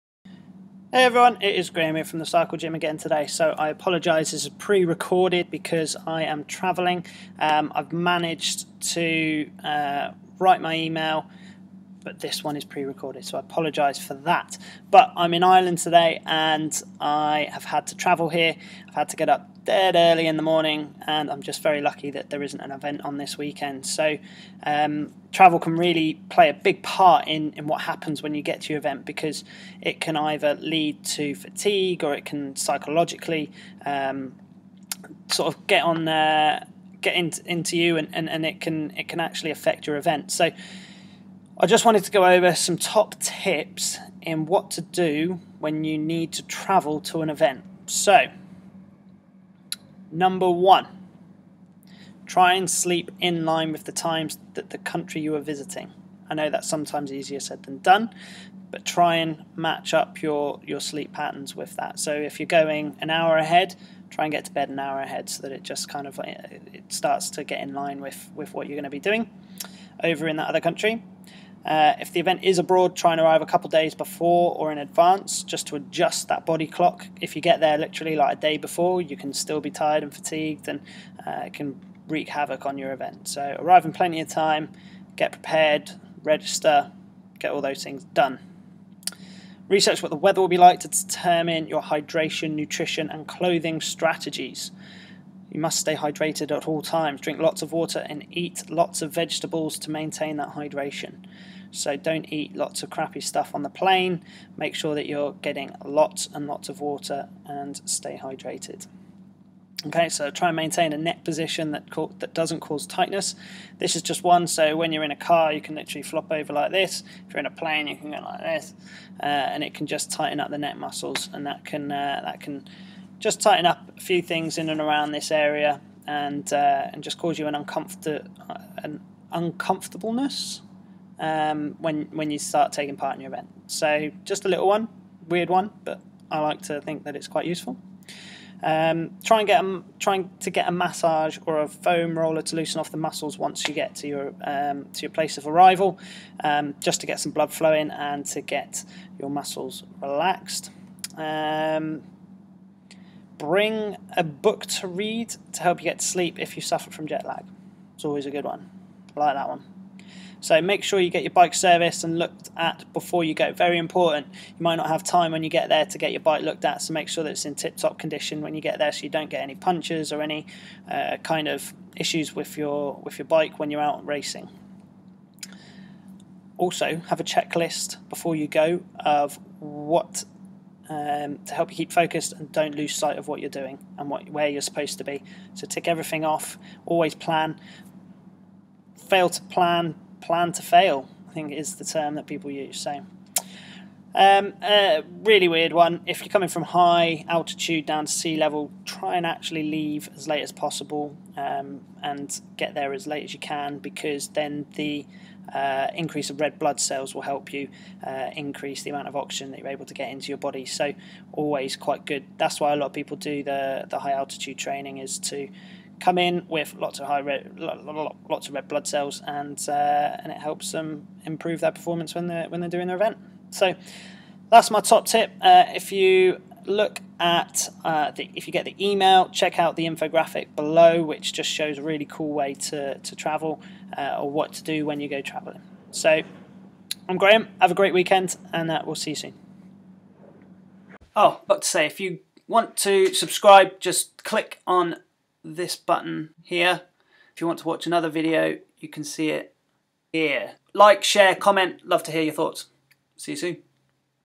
TOP TIPS FOR TRAVELLING TO AN EVENT
Not sure what process to go through when you are travelling to a cycling event? Make sure you watch our quick video which explains exactly what to do when you travel to our event.

Check out our Team Sky Bike Bags for sale on our shop.